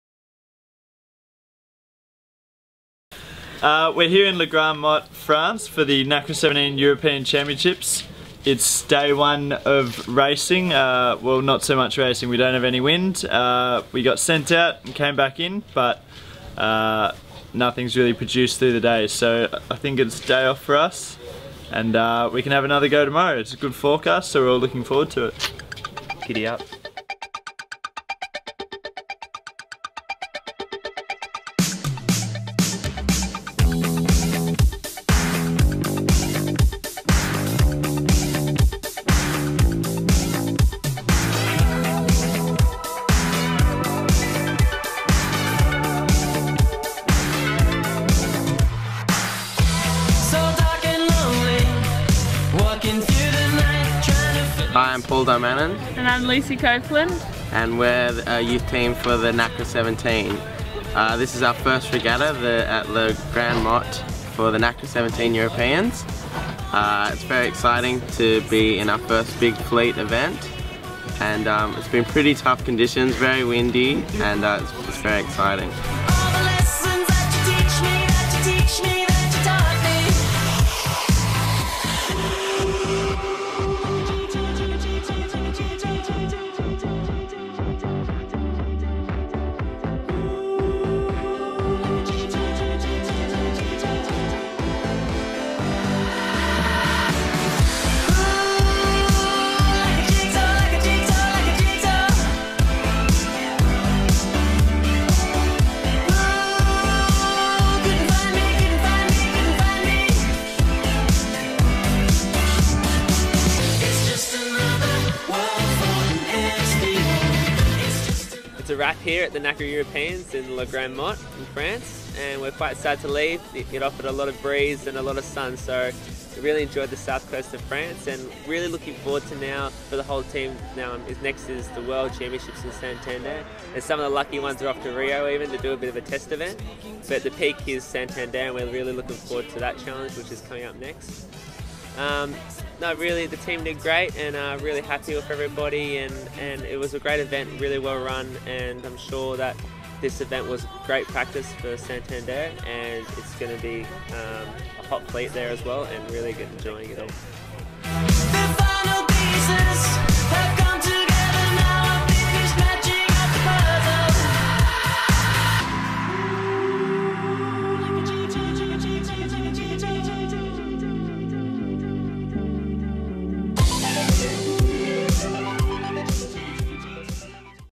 2014 Nacra17 European Championship, La Grande Motte
2014 Nacra17 European Championship Australian Sailing wrap. Check out some great AusSailing racing shots & hear more about the regatta week from our Australian Sailing Squad athletes James Wierzbowski, Paul Darmanin & Lucy Copeland. Australian Sailing Team's Jason Waterhouse (5th with Lisa Darmanin) wraps up the event & tells us what lies ahead.